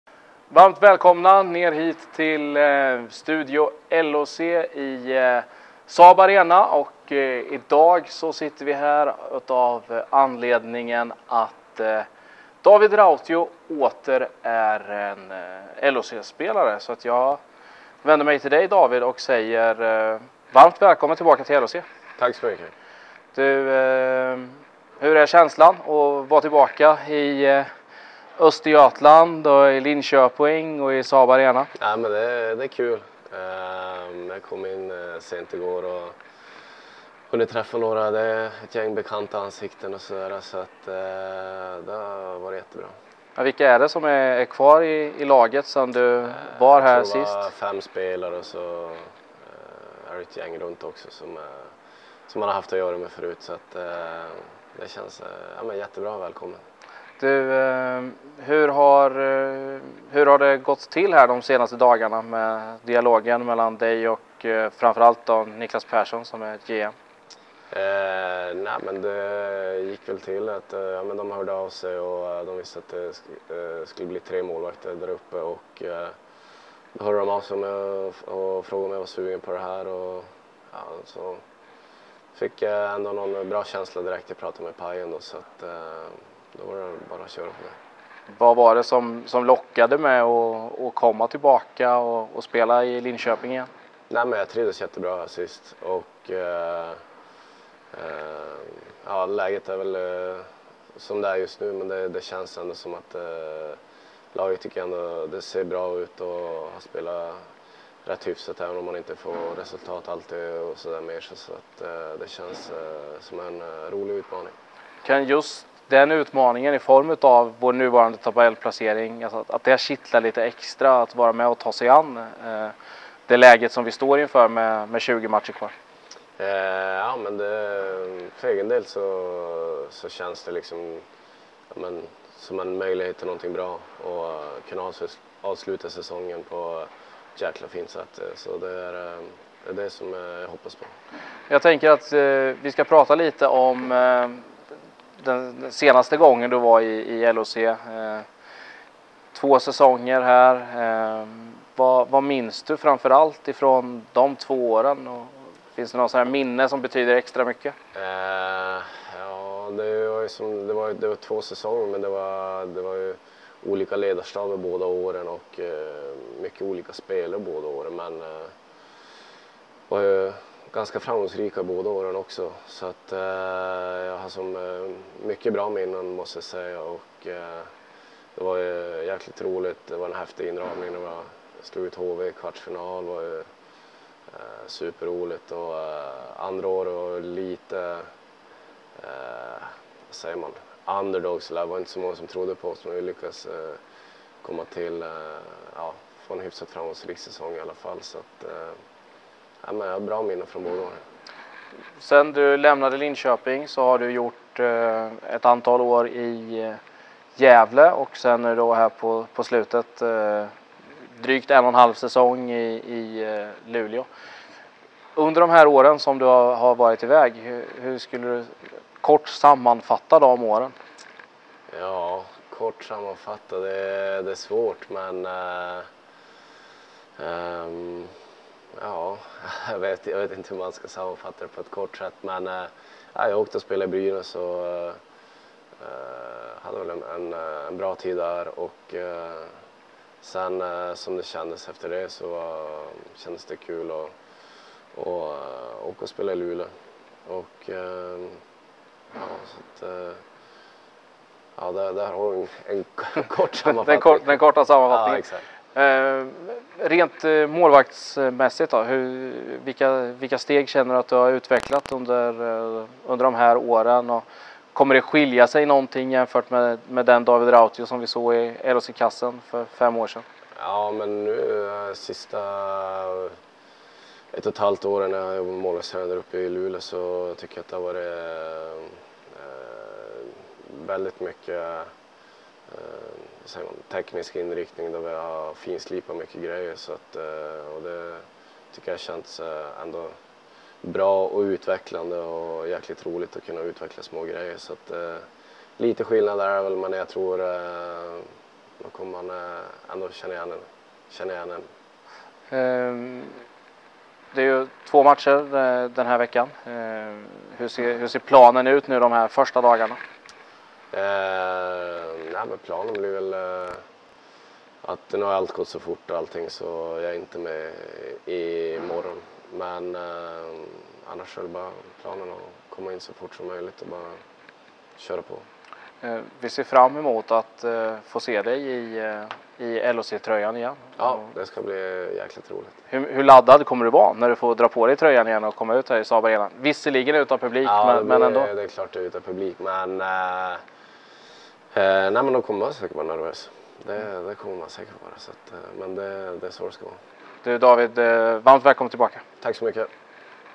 David Rautio: "Jag trivdes jättebra här sist"
Intervju med målvaktsnyförvärvet David Rautio, som idag anslöt till Linköping Hockey Club.